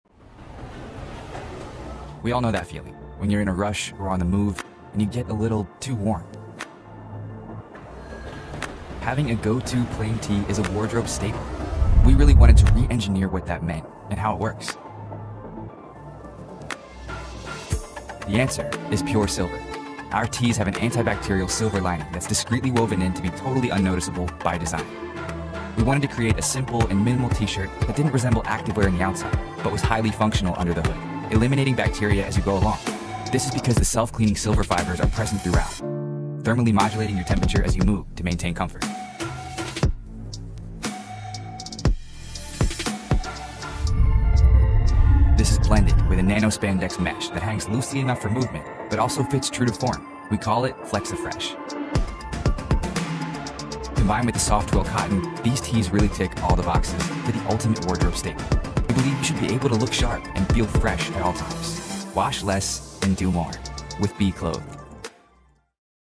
Silver Tees 👕 Self-Cleaning Silver-Infused T-Shirts
Self-cleaning T-shirts made from pure Silver that keep you fresh and free from odour, all day long.

Last video : 3 in 1 Cross Paralel Lifestyle Jacket